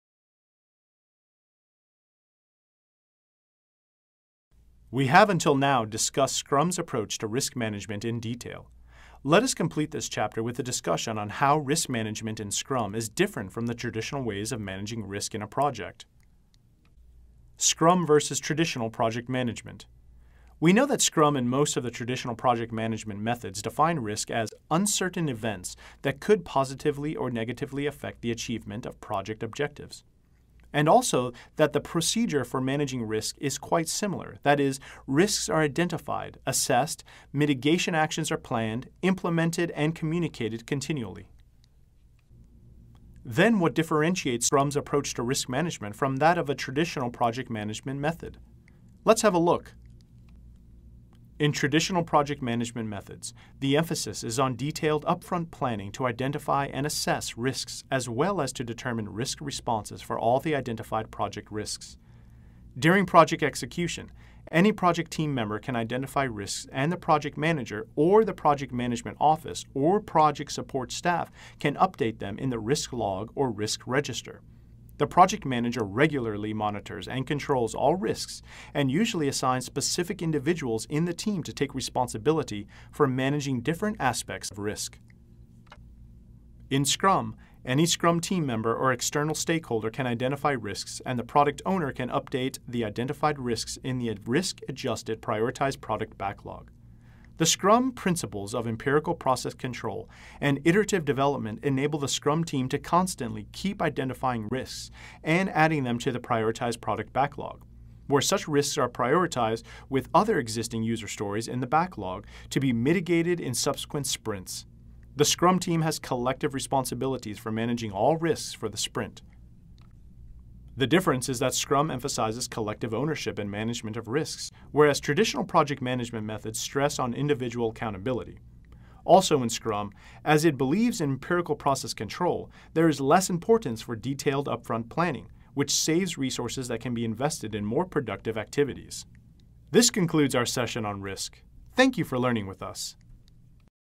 Scrum Developer C07M06 Scrum vs Traditional Project Management
The last module of the Risk Chapter is Scrum vs Traditional Project Management. Scrum and most of the traditional project management methods define risk as 'Uncertain event(s) that could positively or negatively affect the achievement of project objectives.